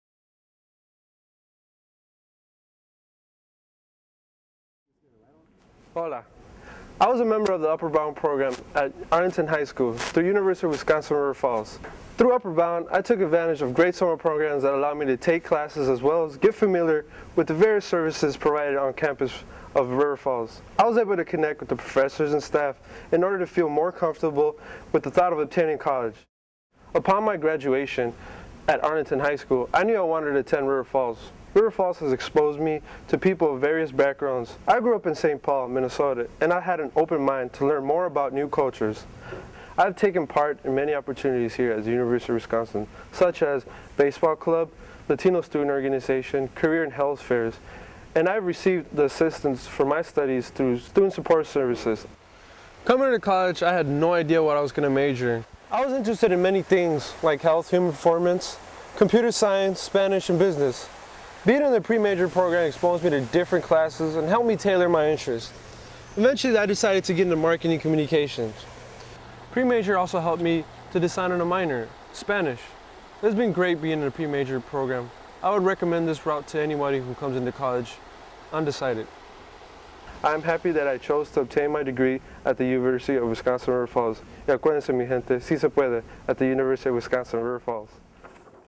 "SUCCESS" - Part 7 "Omar" (Edited by Christopher Kibellus)
SUCCESS- Part 7
A promotion of Academic Success Center and Student Support Services of University of Wisconsin River Falls.  This project was headed and executed by students of UWRF in 2008 and edited by Christopher M Kibellus for the purposes of distribution by the university for the promotion of the programs offered.
This video is displayed on the CKibellusStudios Youtube account with the permission from the overseeing faculty advisor of the project.
Other questions regarding the programs outlined herein should be directed to the proper faculty, staff, and/or divisions which can be found through the university